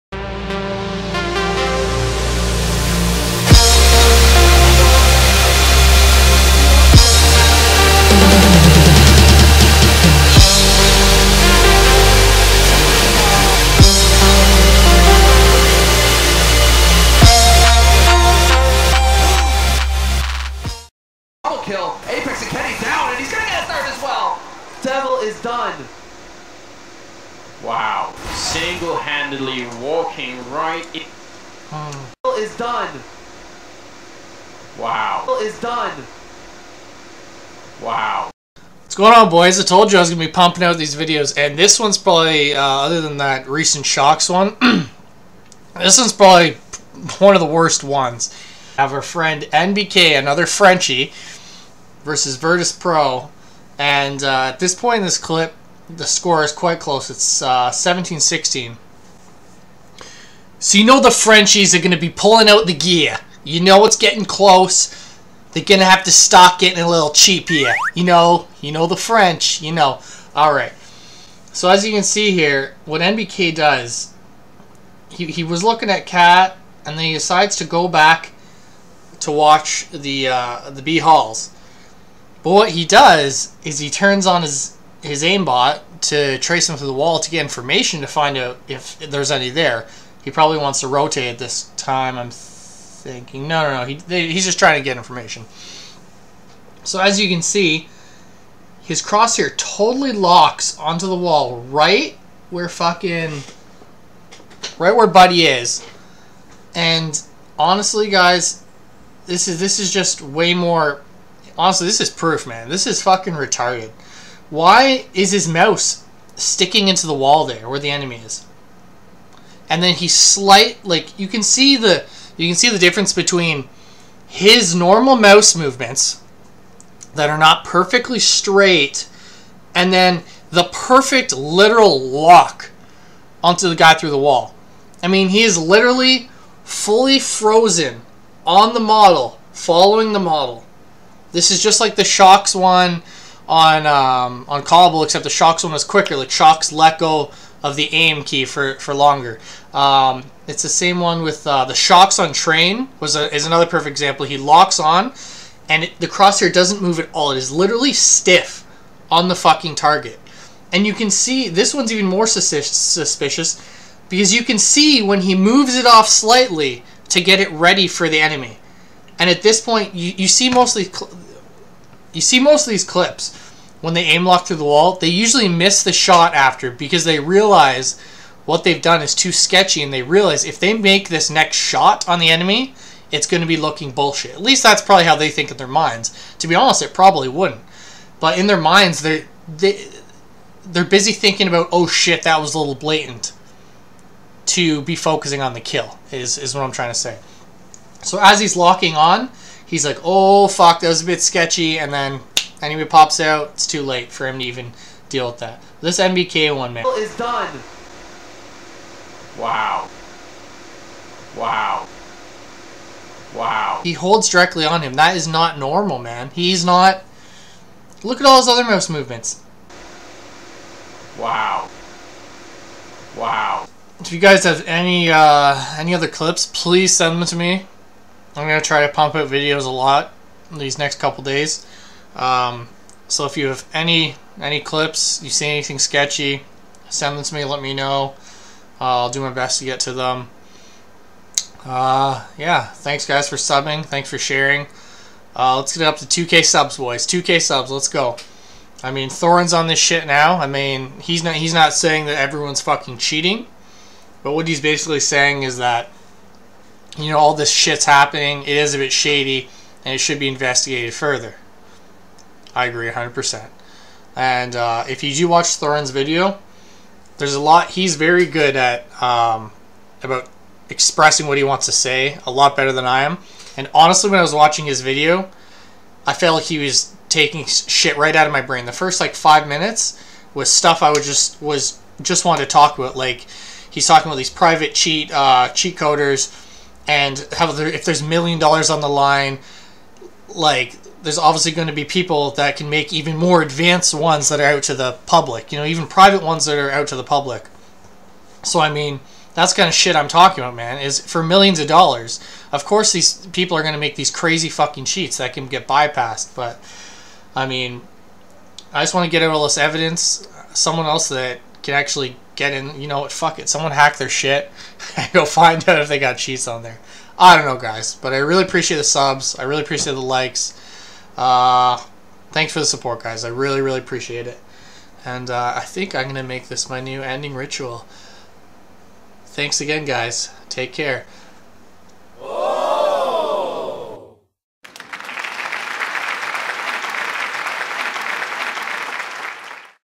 NBK BLATANT AIMBOT lock versus virtus pro
NBK doing a horribly blatant LOCK on vs Virtus pro, I mean this is ridiculous. Most blatant one you have ever seen. Please help make the scene CLEAN!

If you would like to donate me "Mouthe open" skins my trade link is below :

And if you want to donate me here is a link for that :